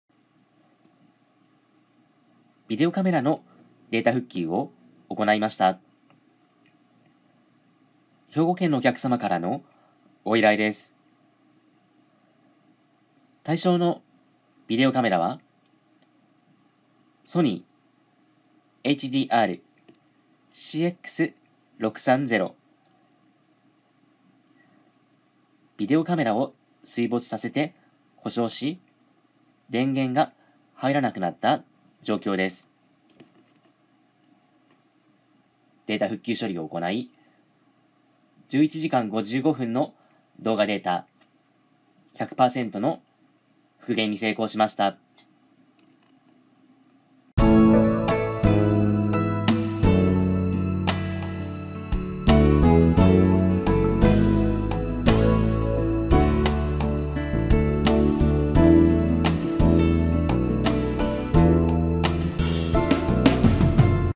SONY ビデオカメラ 電源が入らない データ復旧 HDR-CX630V
SONY ビデオカメラ 電源が入らない データ復旧 HDR-CX630V 兵庫県
水没故障ビデオカメラのデータ復旧。
SONY HDR-CX630Vハンディカムを水没させて故障し、電源が入らない。
データ復旧に成功しました。
故障したビデオカメラのデータ復旧なら、お任せください！

SONY ビデオカメラのデータ復旧【とにかく間違いなし！オススメ！】